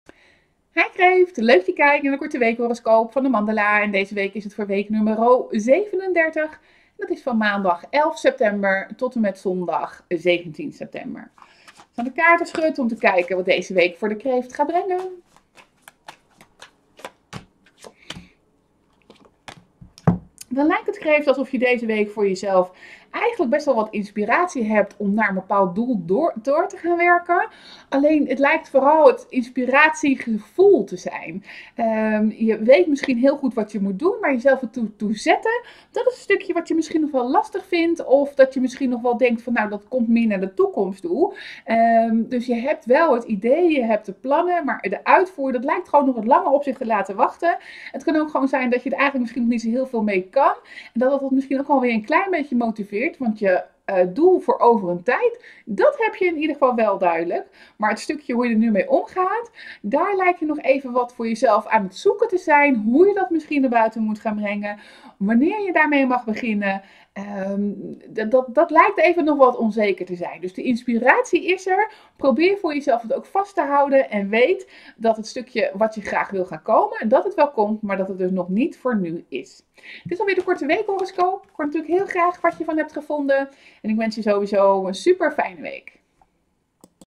Weekhoroscoop Kreeft week 37 2023
Korte tarot weekhoroscoop Kreeft week 37 2023 - maandag 11 september t/m zondag 17 september